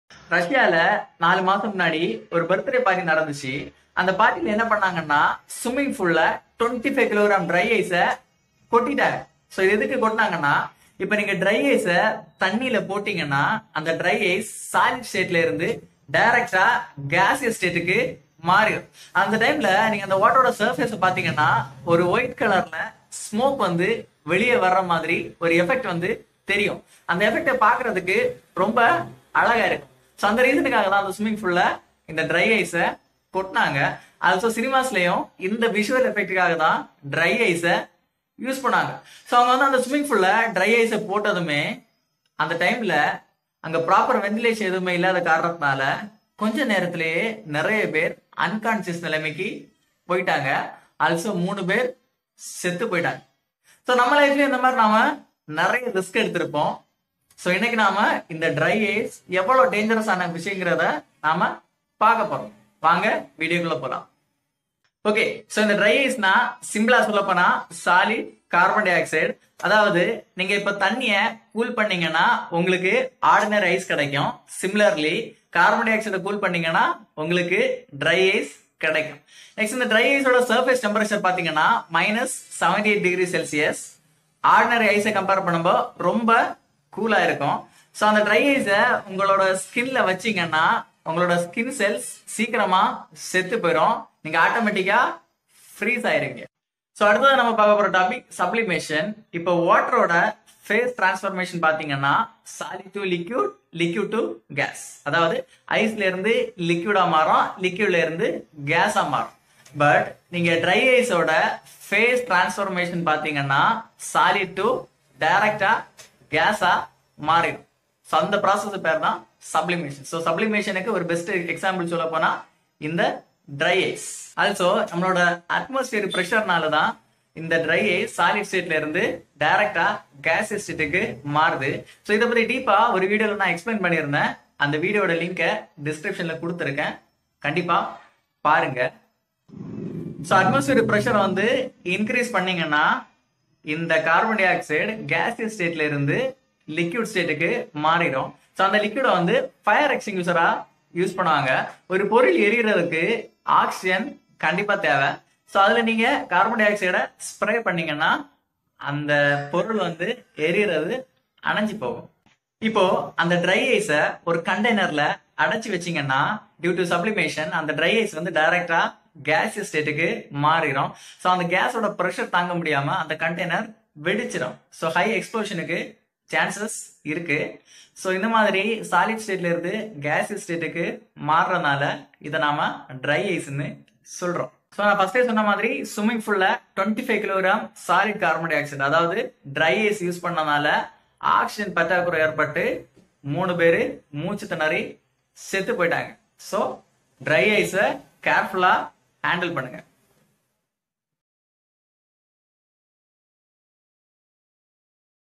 Three people died in russia due to dry ice
in this video we are discussing about  three people have been killed after dry ice was poured into a swimming pool ... Yekaterina Didenko at a pool complex in the Russian capital.